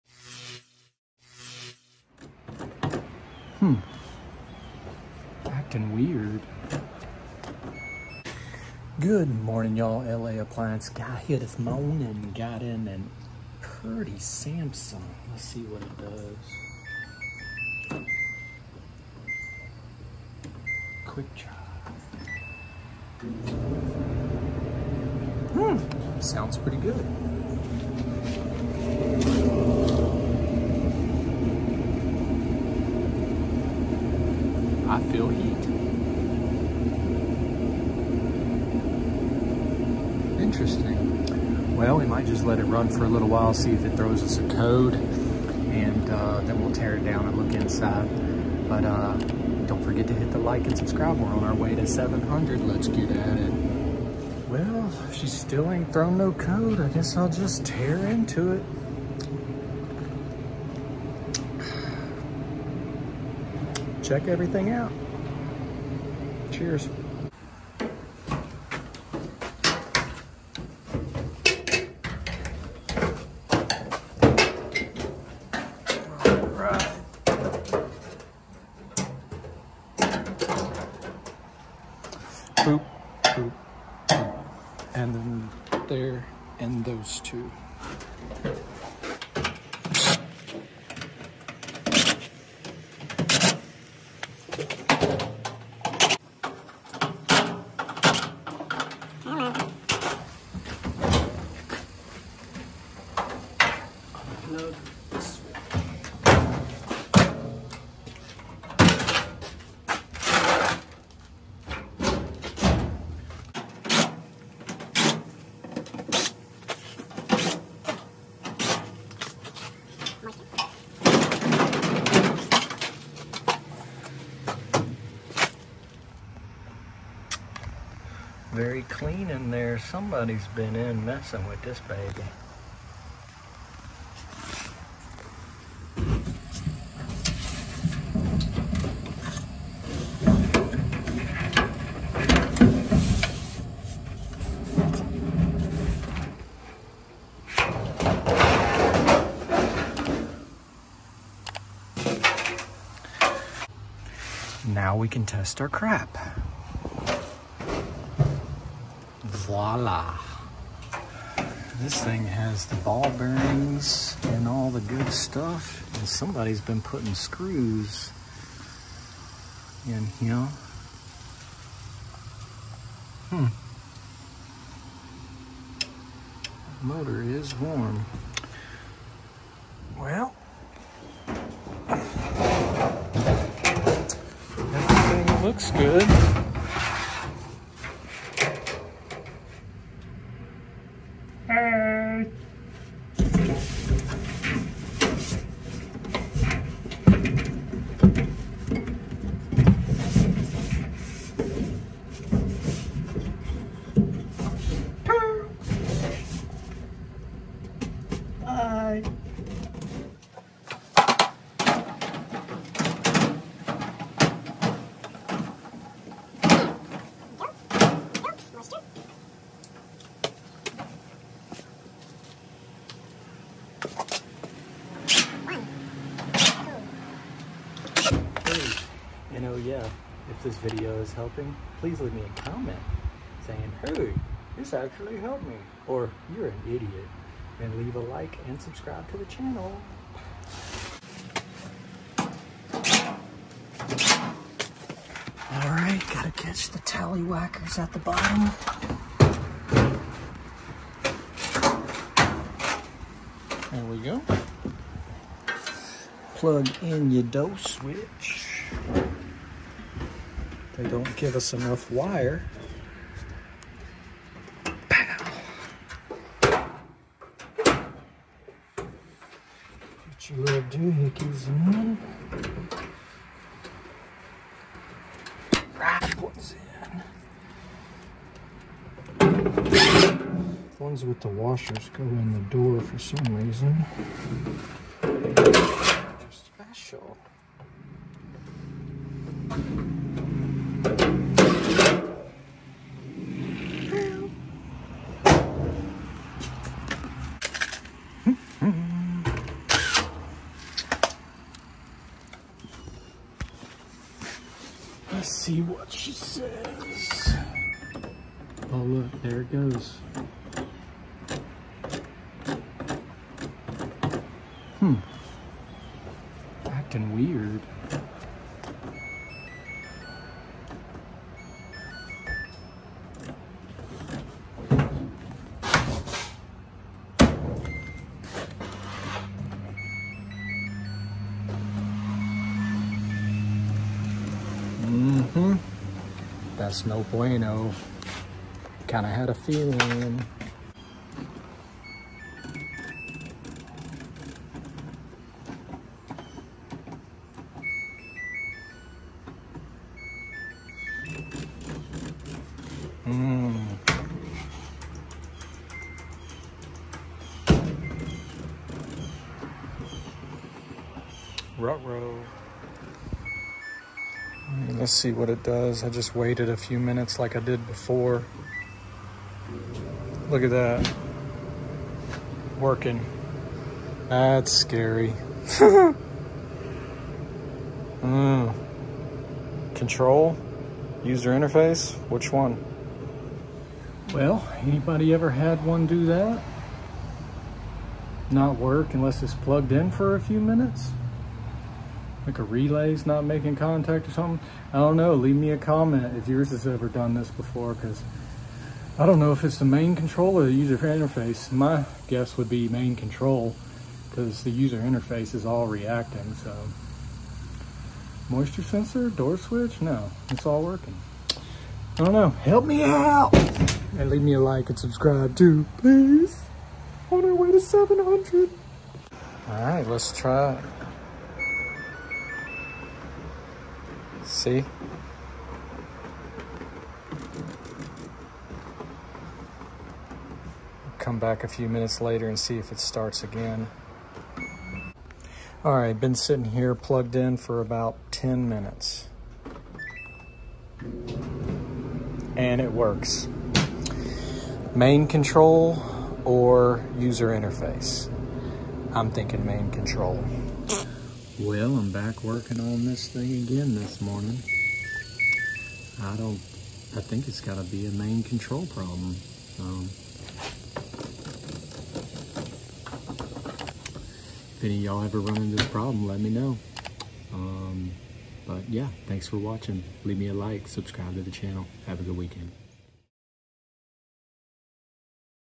SAMSUNG DRYER HELP! It’s got a mind of its own | starts sometimes and not other times
HELP ANYONE HAD THIS PROBLEM BEFORE!?

Got in a Samsung that’s running great sometimes. So I figure it’s a main pcb problem but I’m not sure. DIY at your own risk.

Let’s get the 1k